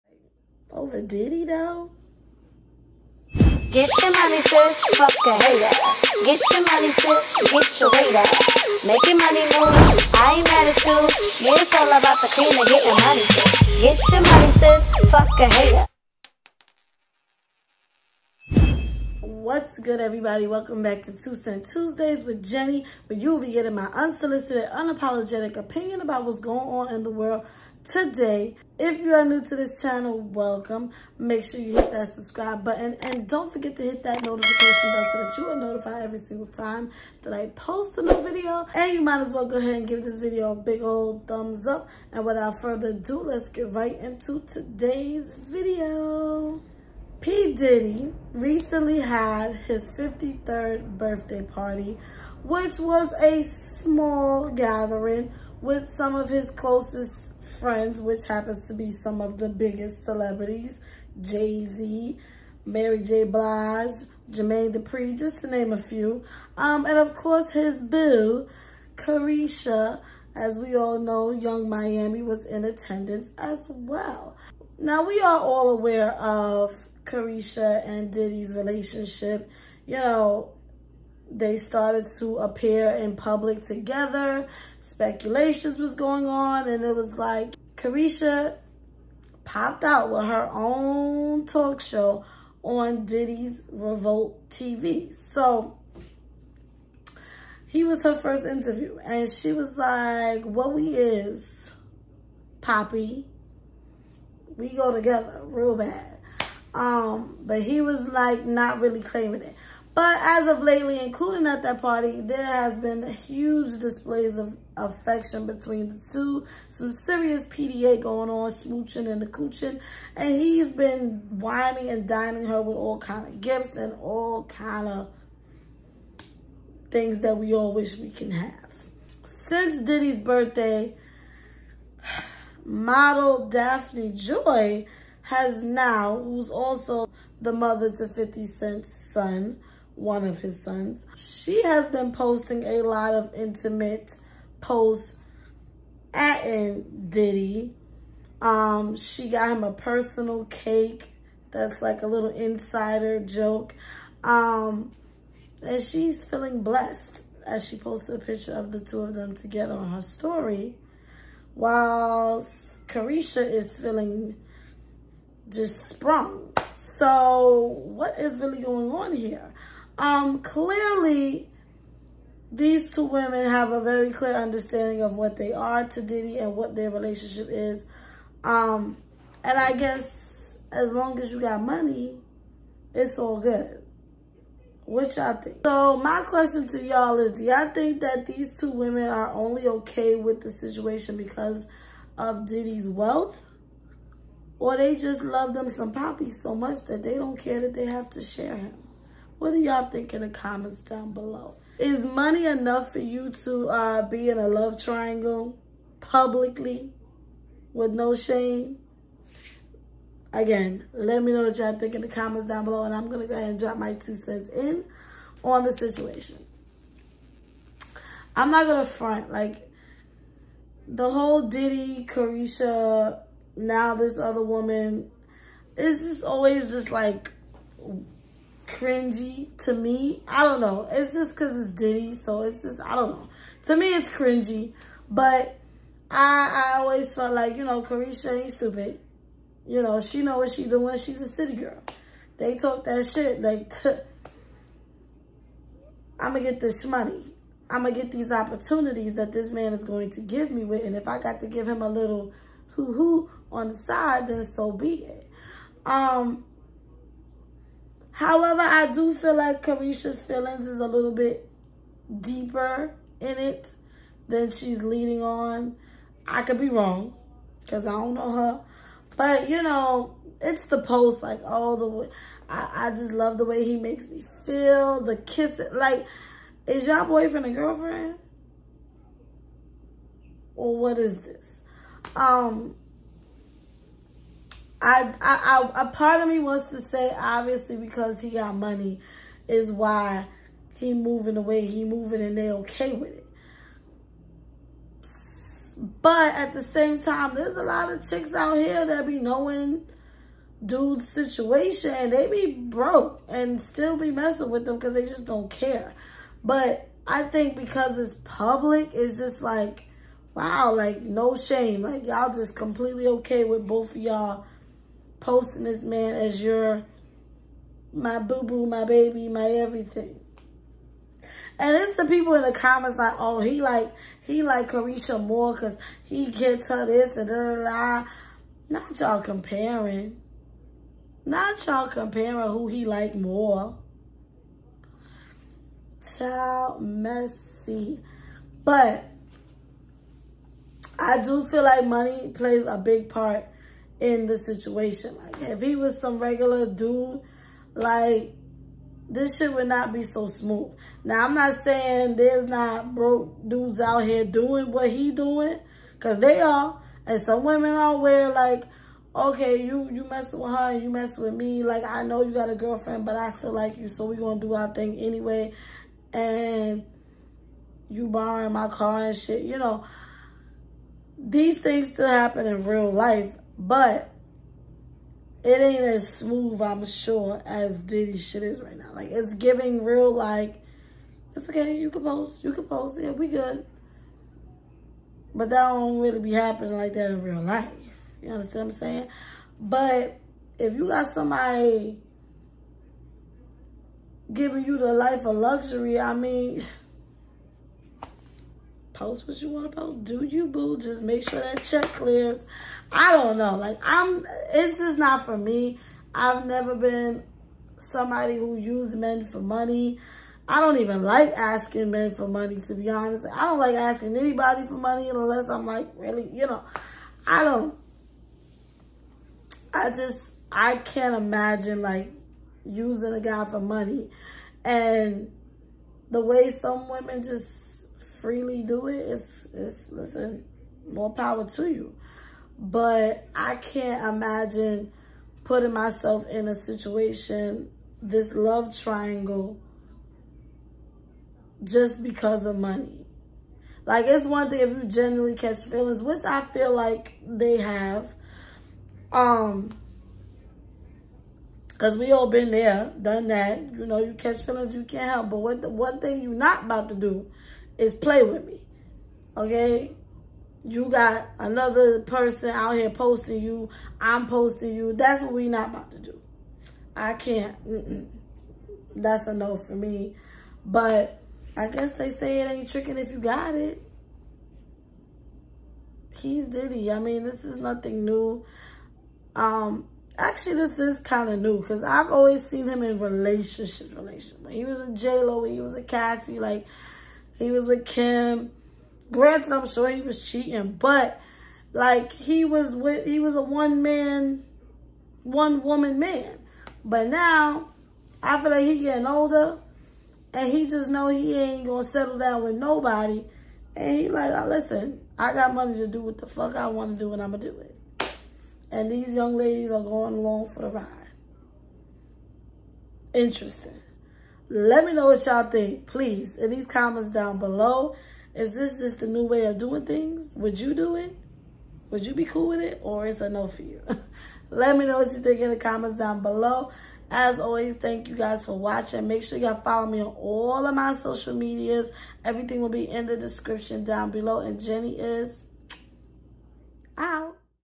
Diddy Dating Young Miami & Daphne Joy | Does Money Make It Acceptable?
Are you willing to be in a polygamy relationship if the person is wealthy?

Tune in EVERY Tuesday

Two Cents Tuesday with Jenny is abt me giving my unsolicited and unapologetic opinions on current topics!